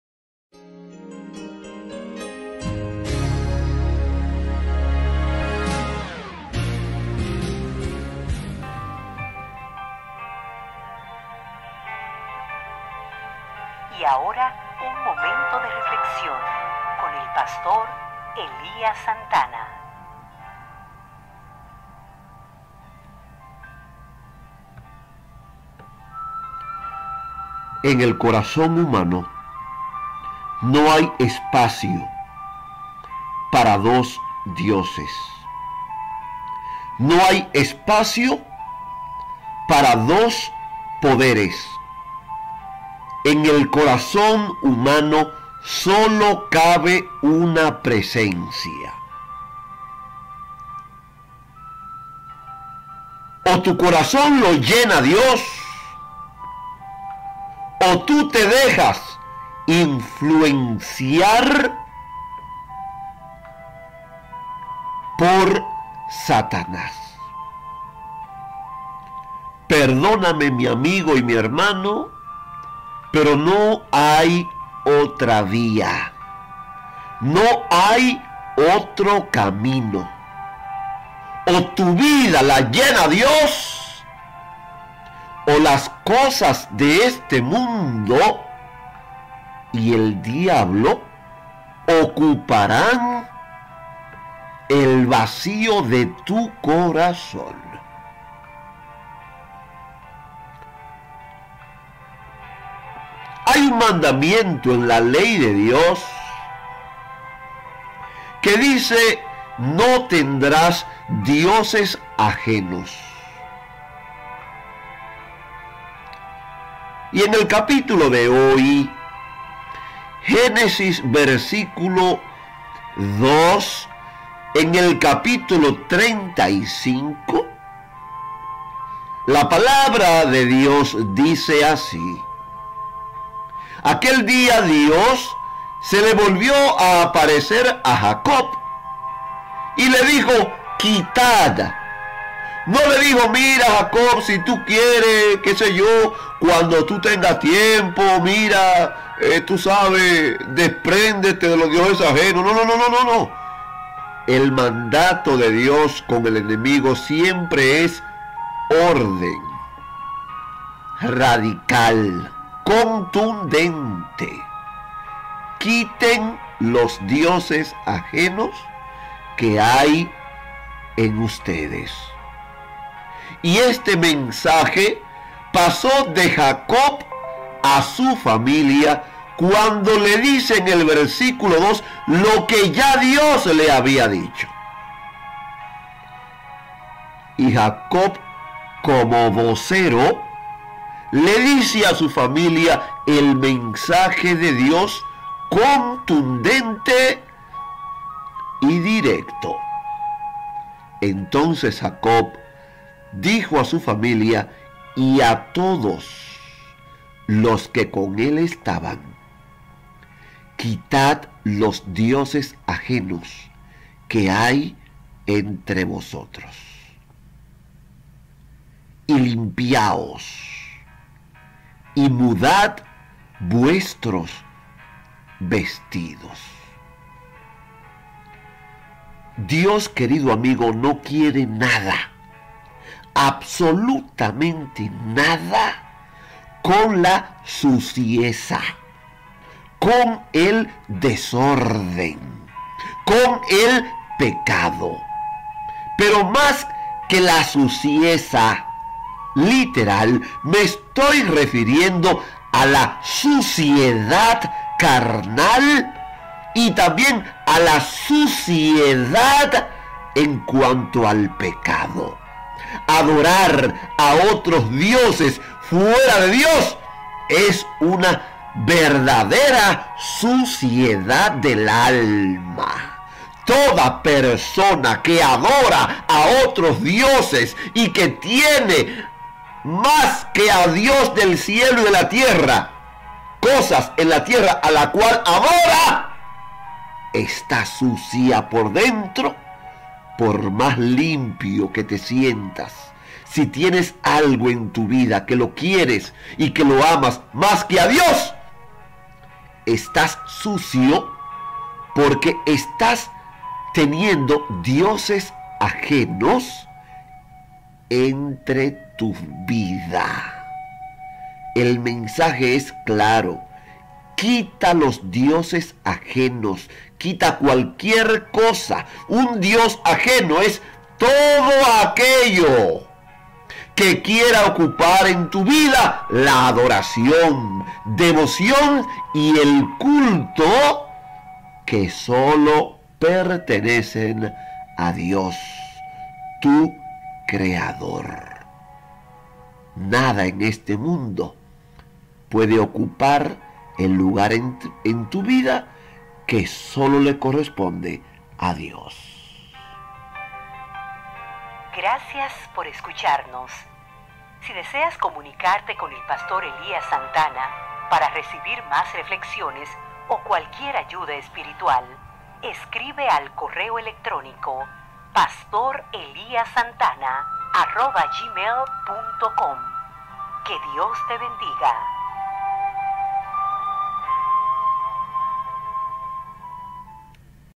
Quitad los dioses ajenos que hay entre vosotros. Génesis 35:2
Un dios ajeno es todo aquello que quiera ocupar en tu vida la adoración, devoción y el culto que sólo pertenecen a Dios, tu creador.